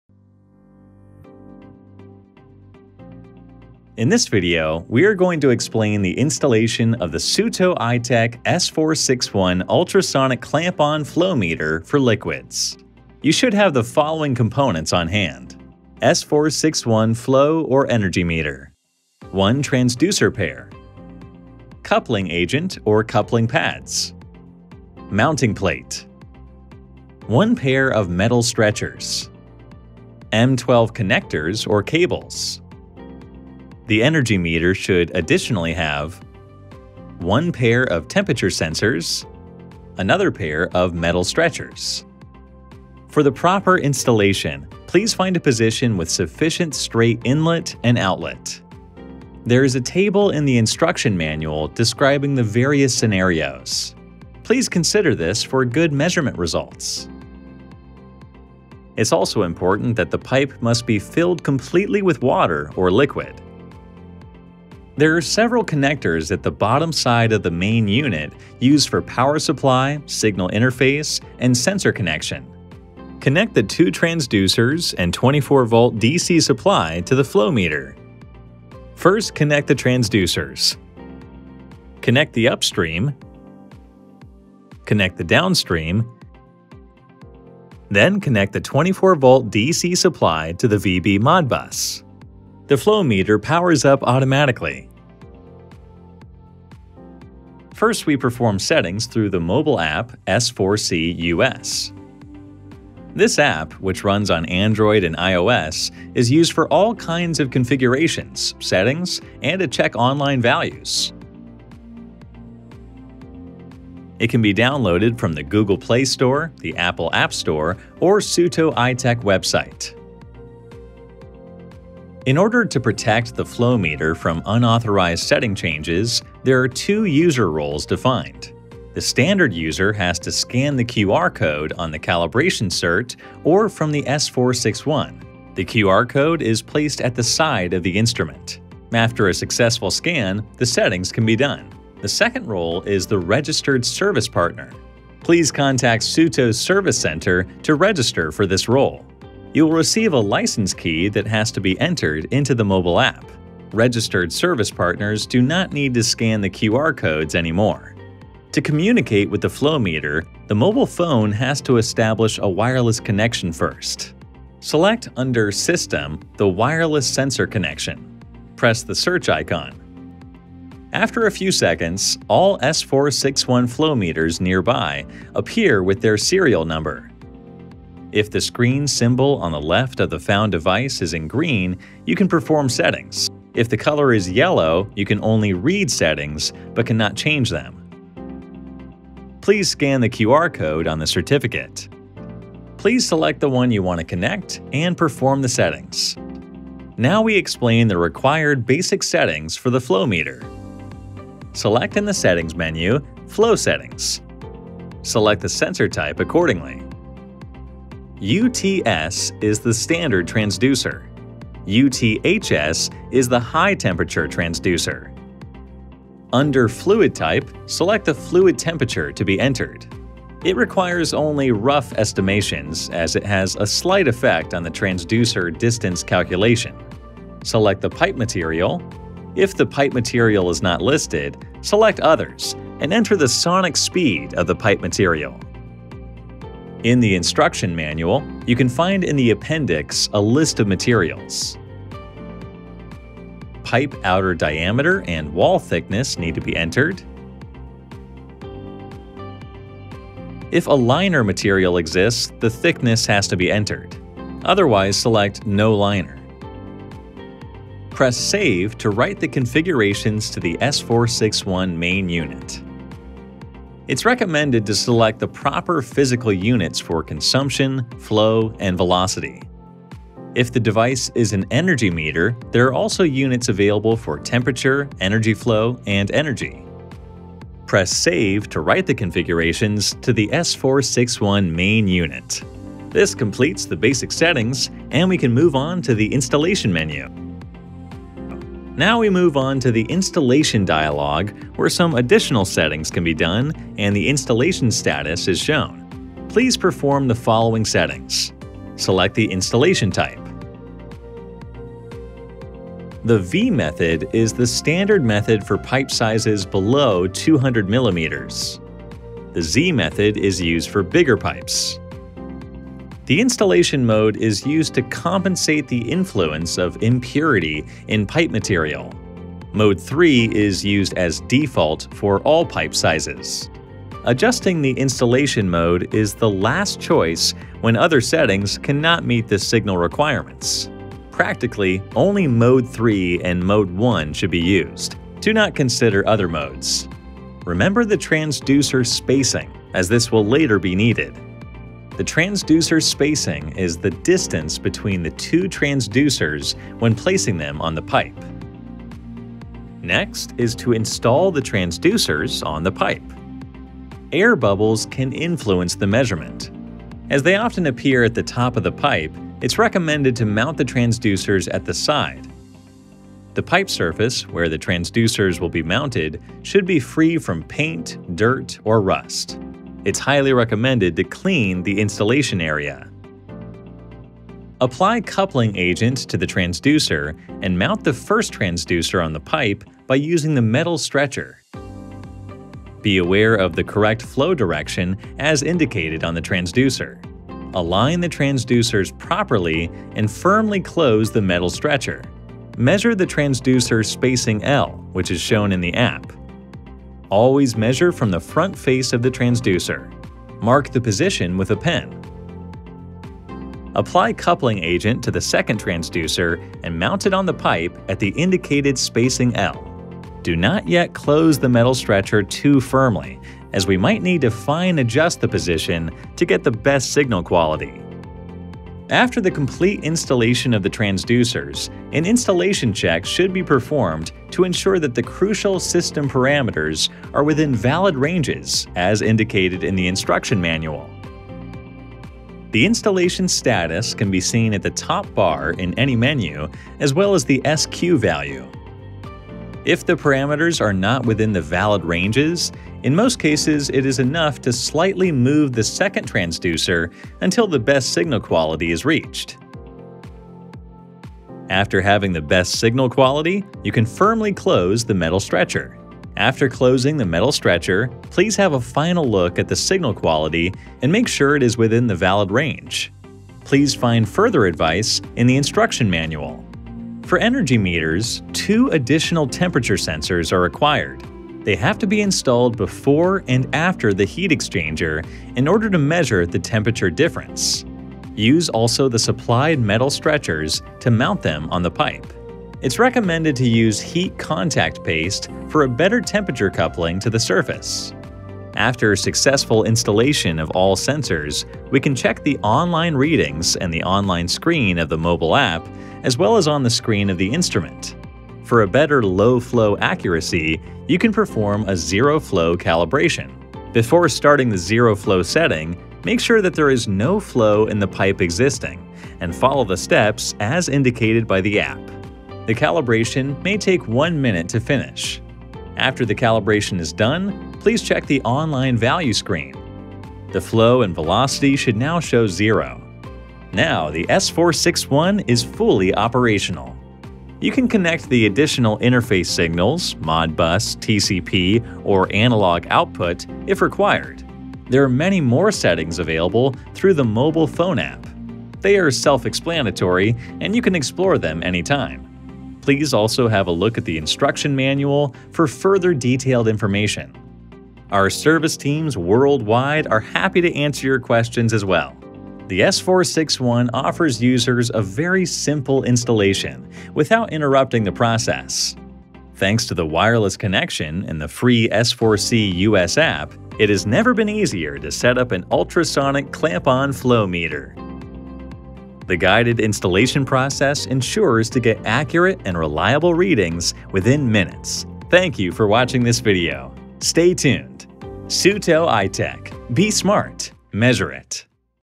S461 Clamp-On Ultrasonic Flow Meter for Water and other Liquids - Installation Video (English)
In this video, we provide detailed instructions on how to configure and install the S461 liquid flow meter. We show the steps to easily configure the device with the associated app and the convenient mounting at the point of use.

Key Features:
- Quick installation and high measuring accuracy
- Measures the actual flow and total consumption of various liquid
- Easy and user-friendly configuration through the dedicated smartphone app
- Additional temperature sensors enables the flow meter to use as energy meter to monitor heat exchangers
- Various signal outputs: Modbus/RTU (standard), 4... 20 mA / Pulse / Alarm-Relay (option), Modbus/TCP (option)
- Flow and consumption can be measured in both directions, forward and reverse (Bi-directional measurement).
- Robust industrial design with versatile installation options for the display unit: Wall installation, DIN rail and pipe installation
- Usage in stationary and portable applications
- Measurement log files can be downloaded through the mobile App, via USB OTG or by using the free S4A Software
- The mobile app offers instant overviews of daily, weekly and monthly consumption at a single click